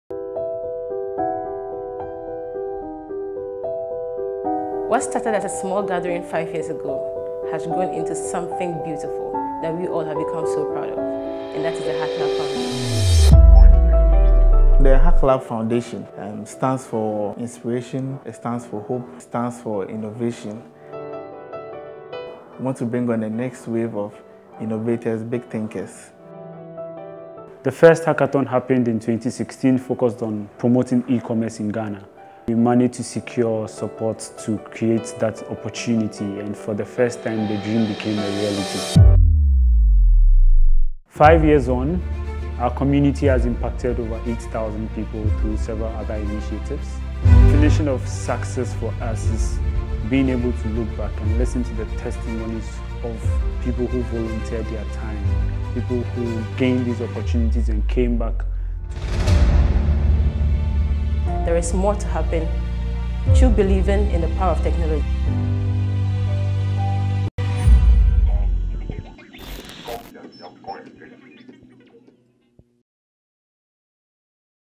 Road To Hacklab 2020 | Official Trailer
5 years ago, we began a journey to equip young Africans with digital skills for future jobs.

Today we have reached over 10,000 of them.

There is more we can do through believing in the power of technology.

Welcome to Hacklab2020.